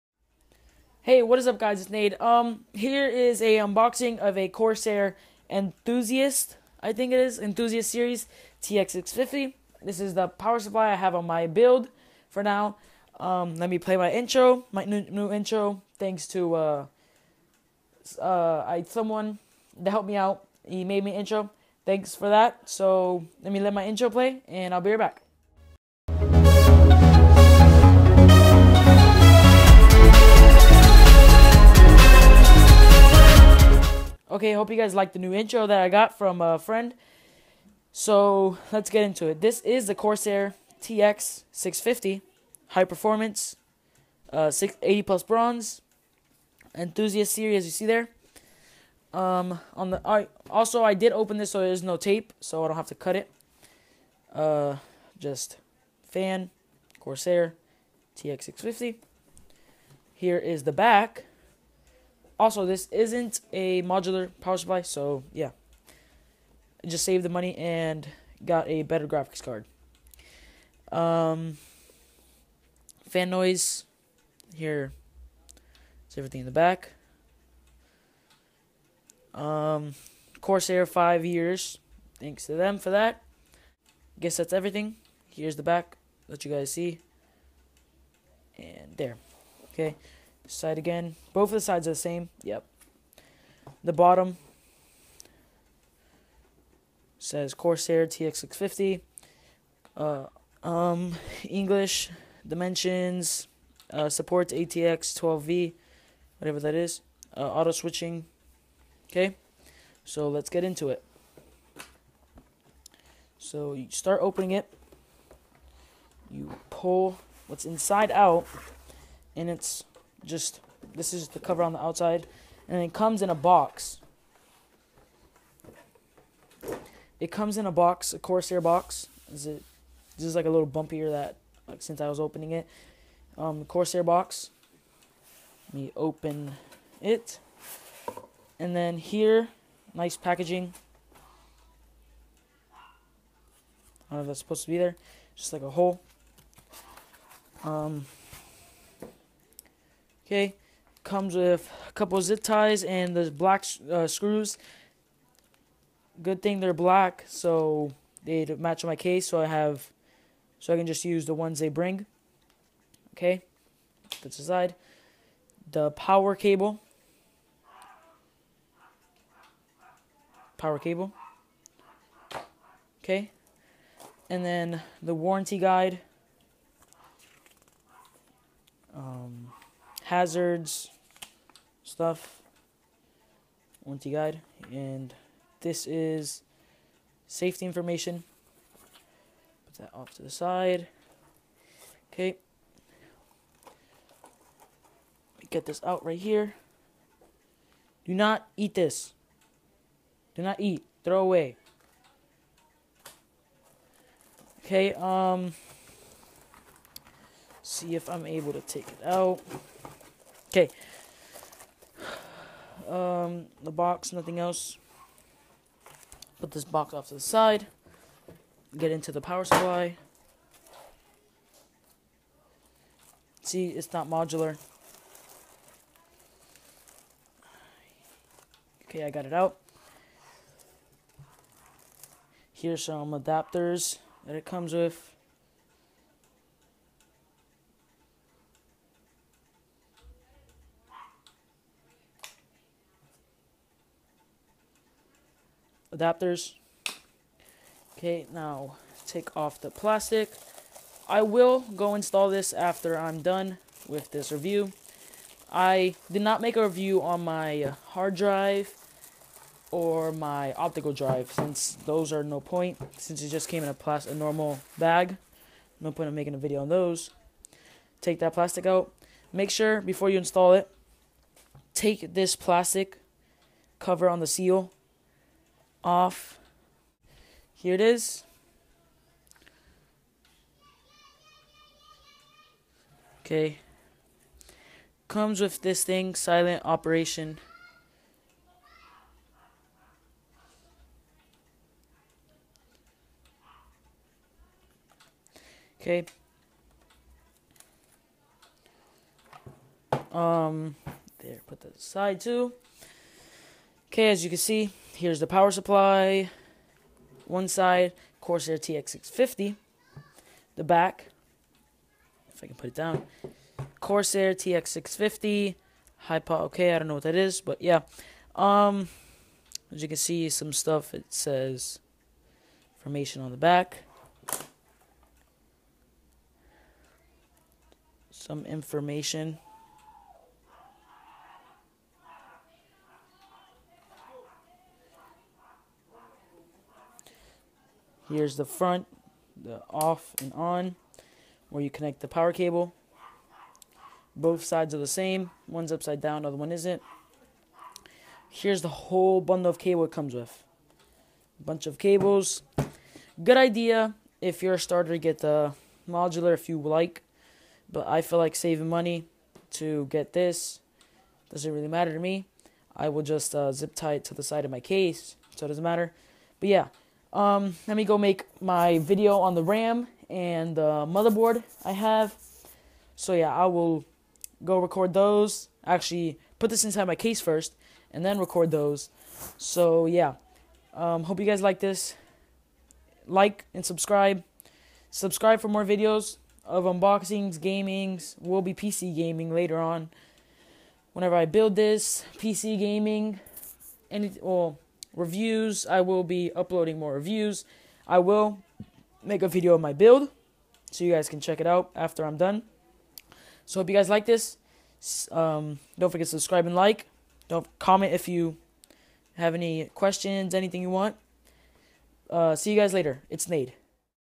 Corsair Tx650 v2 unboxing
Unboxing of a Corsair Tx650 enthiusaist. will be doing unboxing reviews gameplays and whatever videos i think will be good. not doing unboxing on case hdd or optical drive because case is to big and the hdd and od nothing much in the packaging. Hope you like the video.